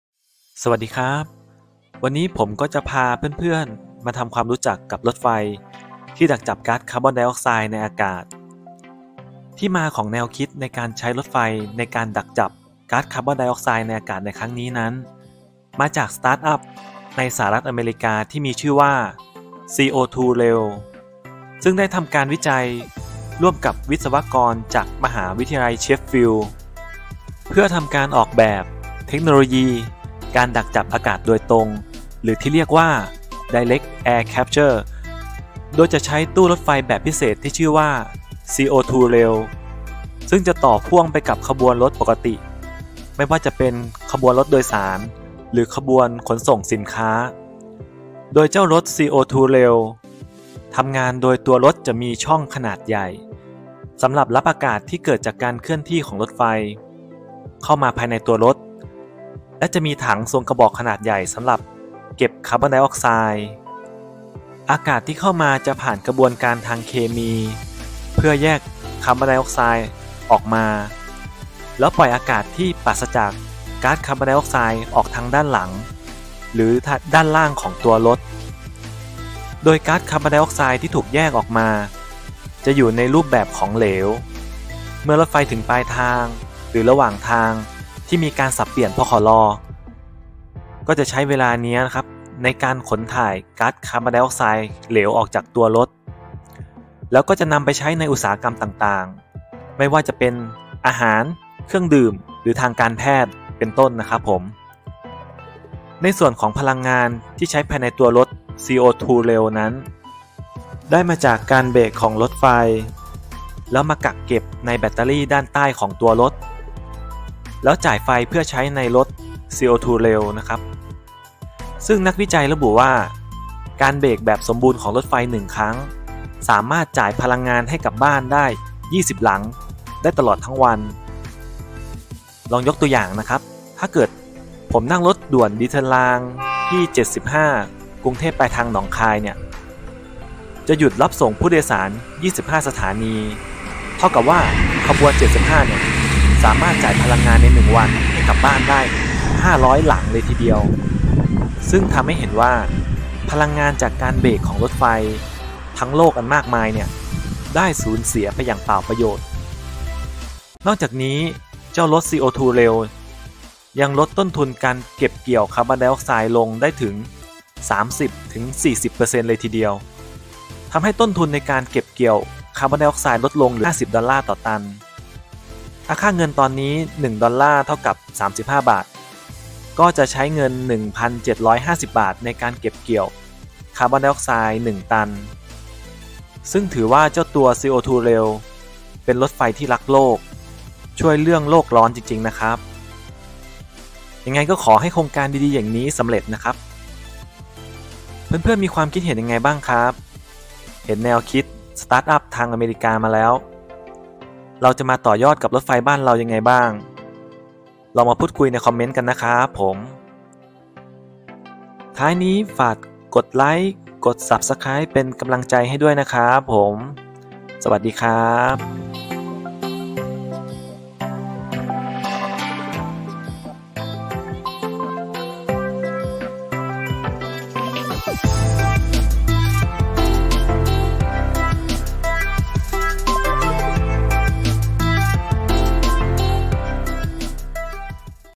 CO2Rail รถไฟดักจับก๊าซคาร์บอนไดออไซด์ | CO2Rail Direct Air Carbon Capture
CO2Rail สตาร์ทอัพจากสหรัฐฯมีแนวคิดที่จะใช้รถไฟในการดักจับก๊าซคาร์บอนไดออกไซด์ แล้วผลิตเป็นคาร์บอนไดออกไซด์เหลว ซึ่งการใช้รถไฟจะช่วยลดต้นทุนในการผลิตลงถึง 30-40%  โดยในำพลังงานจากการเบรกของรถไฟนำมาใช้